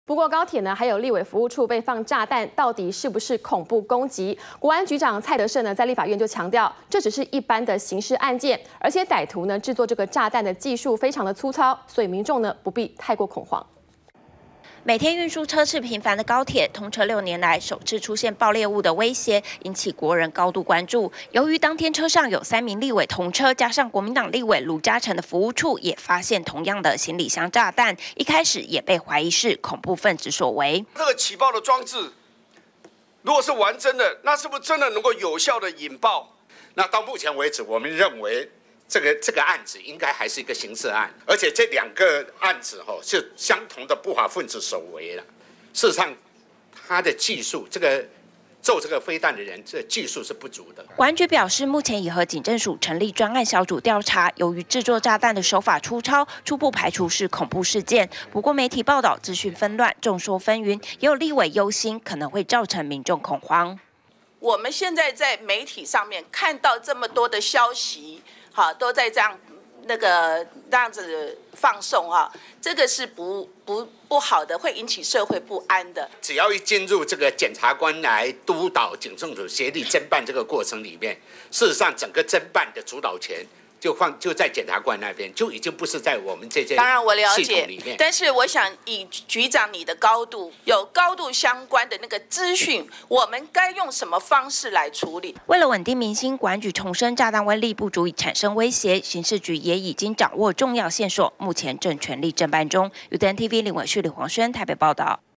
【2013.04.15】國安局長強調 炸彈非恐怖攻擊 -udn tv
高鐵和立委服務處被放置炸彈，到底是不是恐怖攻擊？國安局長蔡得勝在立法院強調，這只是一般的刑事案件，而且歹徒製作炸彈的技術粗糙，民眾不必太過恐慌。